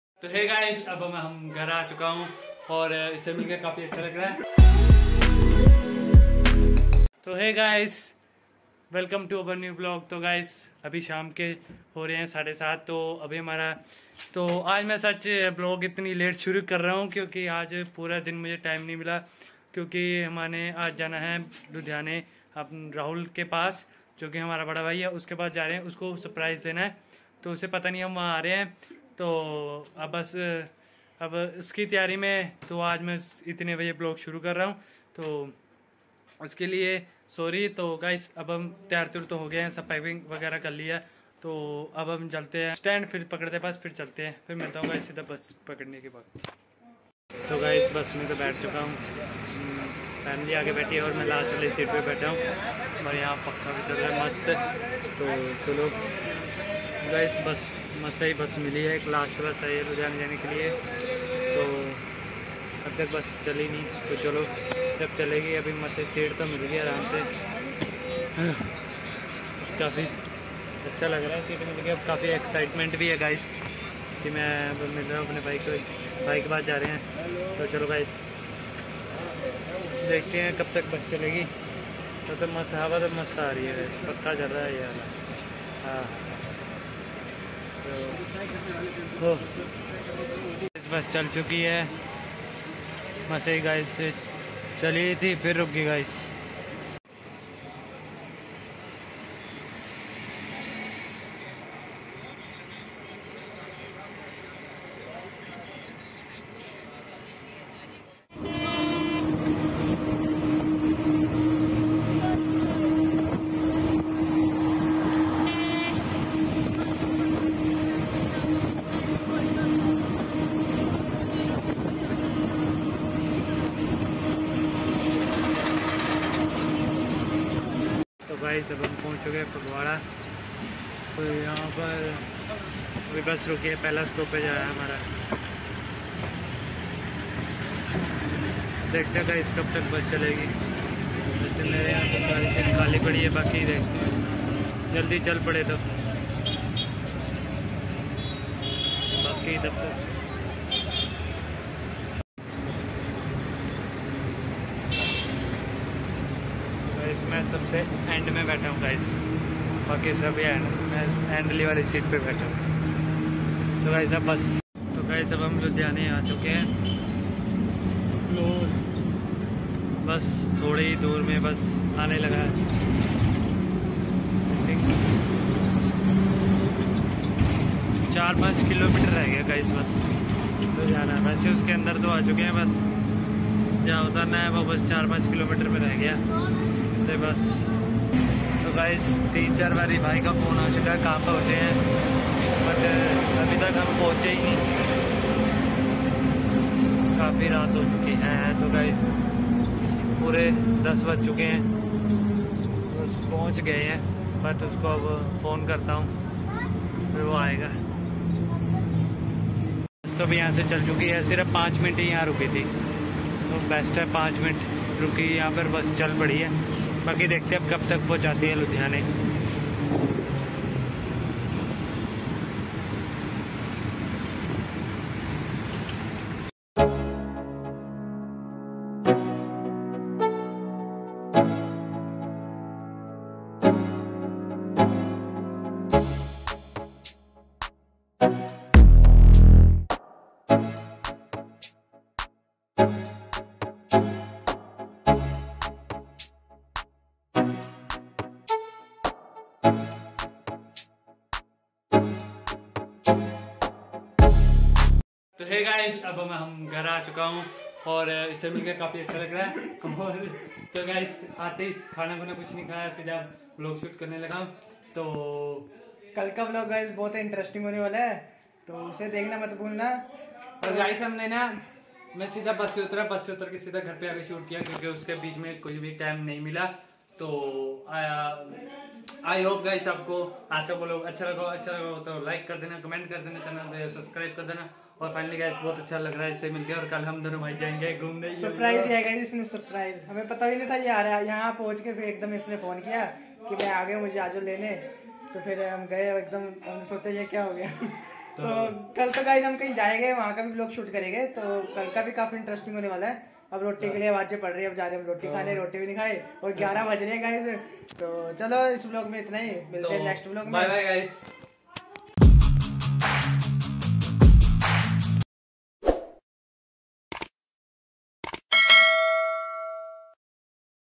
#42 VLOG 🙏🙏 Raat ko 10:30 bje pahuch gye Ludhiana (tired journey)🥱🥴
Hello guys I hope you all are well and Fully enjoyed our this vlog👍 Follow me on: Instagram ID